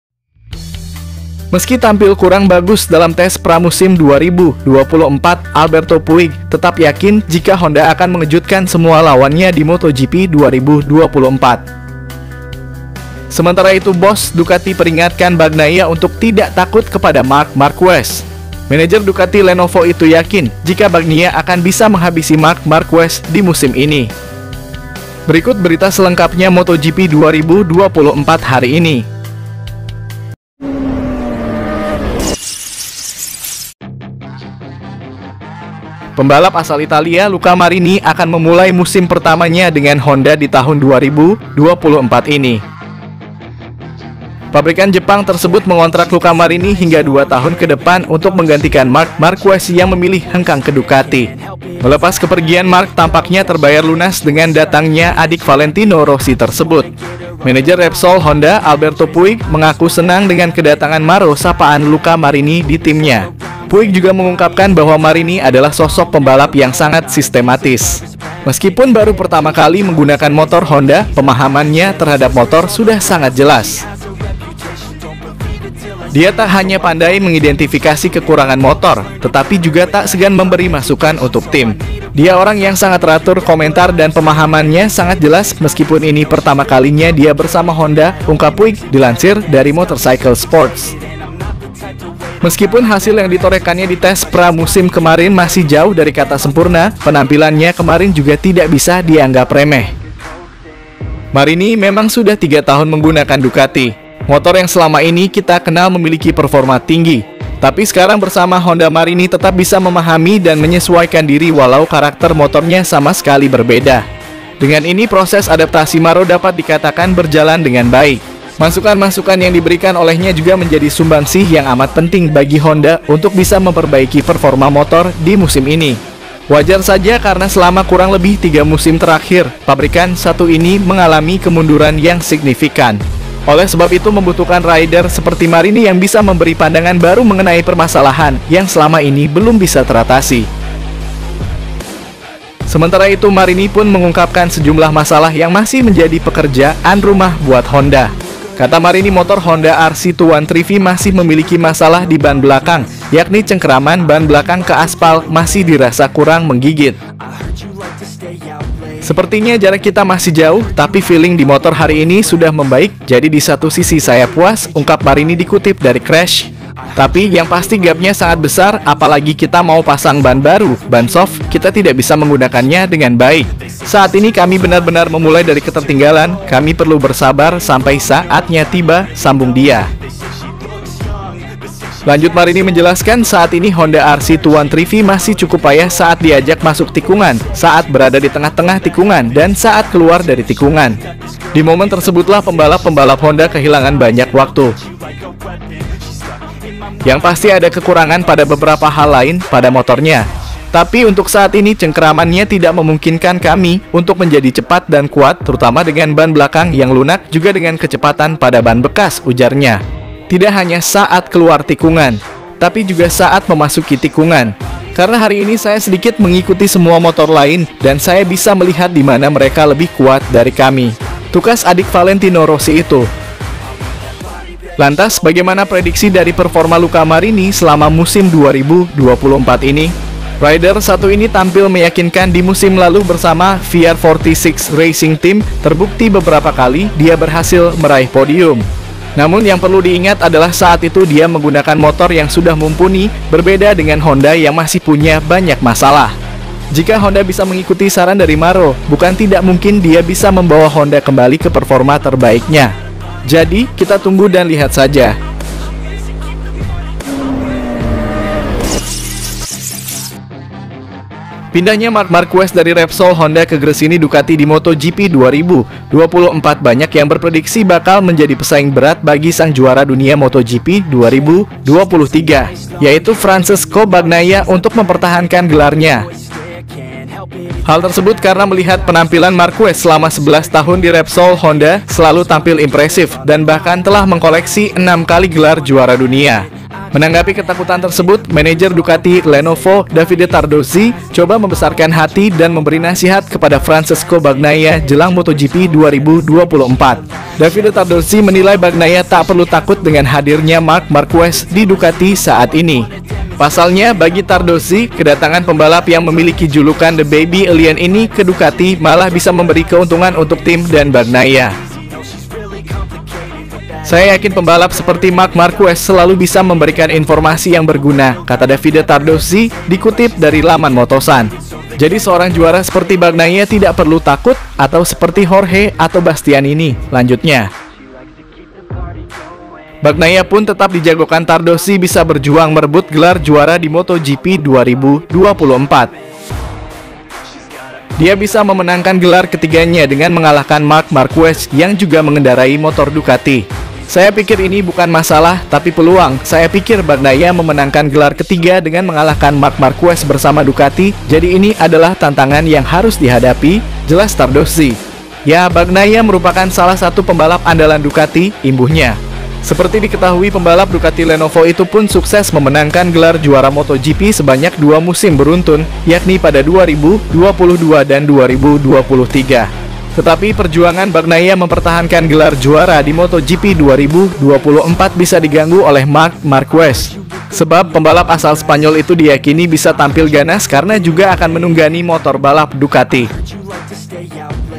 RACE MOTOGP QATAR 2024, LUCA MARINI SEMBUNYIKAN KECEPATAN HONDA RC213V 2024, MOTOGP HARI INI
Pembalap asal Italia, Luca Marini, akan memulai musim pertamanya dengan Honda di tahun 2024 ini. Pabrikan Jepang tersebut mengontrak Luca hingga dua tahun ke depan untuk menggantikan Marc Marquez yang memilih hengkang ke Ducati.

Melepas kepergian Marc tampaknya terbayar lunas dengan datangnya adik Valentino Rossi tersebut. Manajer Repsol Honda, Alberto Puig, mengaku senang dengan kedatangan Maro (sapaan Luca Marini) di timnya.

Puig juga mengungkapkan bahwa Maro adalah sosok pembalap yang sangat sistematis. Meskipun baru pertama kali menggunakan motor Honda, pemahamannya terhadap motor sudah sangat jelas.

Dia tak hanya pandai mengidentifikasi kekurangan motor, tetapi juga tak segan memberi masukan untuk tim.

"Dia orang yang sangat teratur. Komentar dan pemahamannya sangat jelas meskipun ini pertama kalinya dia bersama Honda," ungkap Puig, dilansir dari laman Motorcycle Sports.

Meskipun hasil yang ditorehkannya di tes pra-musim kemarin masih jauh dari kata sempurna, penampilannya kemarin juga tidak bisa dianggap remeh.

Marini memang sudah tiga tahun menggunakan Ducati, motor yang selama ini kita kenal memiliki performa tinggi, tapi sekarang bersama Honda Maro tetap bisa memahami dan menyesuaikan diri walau karakter motornya sama sekali berbeda. Dengan ini, proses adaptasi Maro dapat dikatakan berjalan dengan baik.

Masukan-masukan yang diberikan olehnya juga menjadi sumbangsih yang amat penting bagi Honda untuk bisa memperbaiki performa motor di musim ini.

Wajar saja karena selama kurang lebih 3 musim terakhir, pabrikan satu ini mengalami kemunduran yang signifikan. Oleh sebab itu, membutuhkan rider seperti Maro yang bisa memberi pandangan baru mengenai permasalahan yang selama ini belum bisa teratasi.